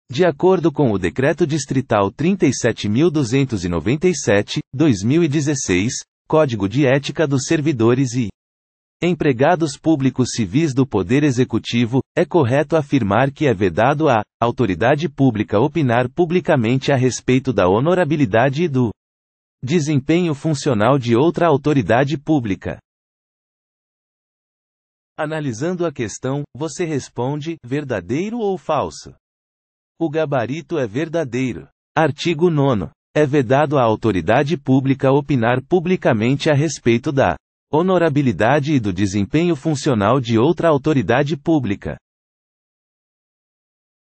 QScon Direito: [Art. 9º, Anexo I, Decreto Distrital 37.297/2016] Texto de Lei
Art. 9º, Anexo I, Decreto Distrital 37.297/2016 | Texto de Lei

De acordo com o Decreto Distrital 37.297/2016 (Código de Ética dos Servidores e. Empregados Públicos Civis do Poder Executivo), é correto afirmar que é vedado à. autoridade pública opinar publicamente a respeito da honorabilidade e do. desempenho funcional de outra autoridade pública.

Gabarito: Verdadeiro